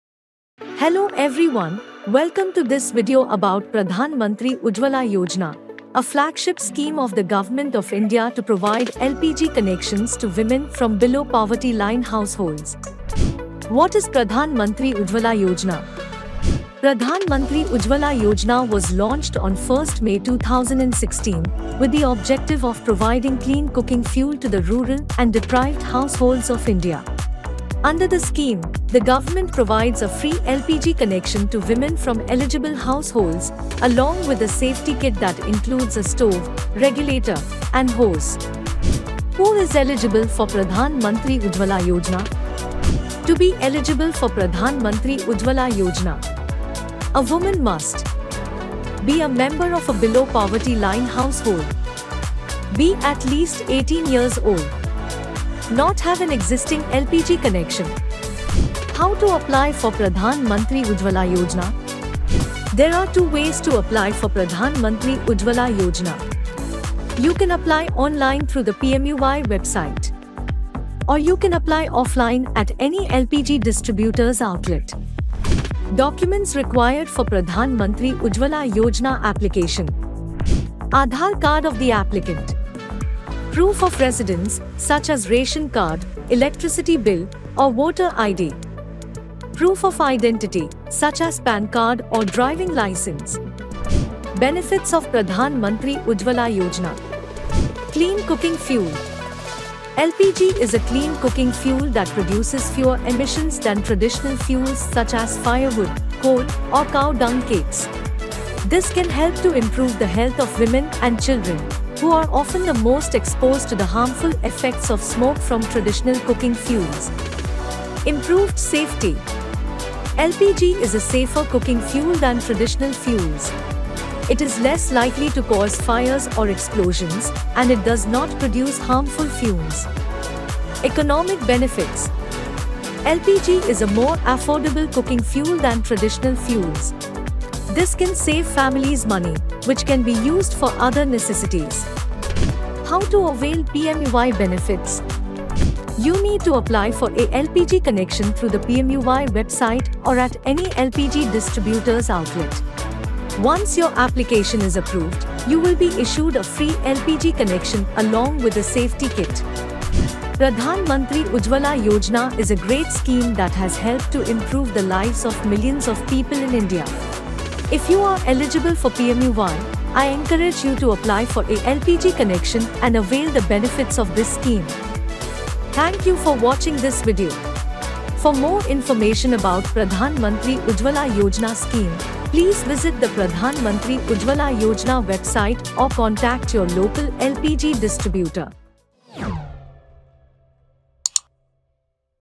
Pradhan Mantri Ujjwala Yojana 2023 | Ujjwala Yojana Free Gas Connection Online Apply | PMUY 2023
Pradhan Mantri Ujjwala Yojana (PMUY) is a flagship scheme of the Government of India to provide LPG connections to women from Below Poverty Line (BPL) households.

What is PMUY?

PMUY was launched on May 1, 2016, with the objective of providing clean cooking fuel to the rural and deprived households of India. Under the scheme, the government provides a free LPG connection to women from eligible households, along with a safety kit that includes a stove, regulator, and hose.

Who is eligible for PMUY?

To be eligible for PMUY, a woman must:

Be a member of a Below Poverty Line (BPL) household

Be at least 18 years old

Not have an existing LPG connection

How to apply for PMUY?

There are two ways to apply for PMUY:

Online: You can apply online through the PMUY website.

Offline: You can apply offline at any LPG distributor's outlet.

Documents required for PMUY application

The following documents are required for PMUY application:

Aadhaar card of the applicant

Proof of residence (such as ration card, electricity bill, or voter ID)

Proof of identity (such as PAN card or driving license)

Benefits of PMUY

There are many benefits to getting a PMUY connection, including:

Clean cooking fuel: LPG is a clean cooking fuel that produces fewer emissions than traditional fuels such as firewood, coal, or cow dung cakes. This can help to improve the health of women and children, who are often the most exposed to the harmful effects of smoke from traditional cooking fuels.

Improved safety: LPG is a safer cooking fuel than traditional fuels. It is less likely to cause fires or explosions, and it does not produce harmful fumes.

Economic benefits: LPG is a more affordable cooking fuel than traditional fuels. This can save families money, which can be used for other necessities.

How to avail PMUY benefits?

To avail PMUY benefits, you need to apply for a LPG connection through the PMUY website or at any LPG distributor's outlet. Once your application is approved, you will be issued a free LPG connection along with a safety kit.

PMUY is a great scheme that has helped to improve the lives of millions of people in India. If you are eligible for PMUY, I encourage you to apply for a LPG connection and avail the benefits of this scheme.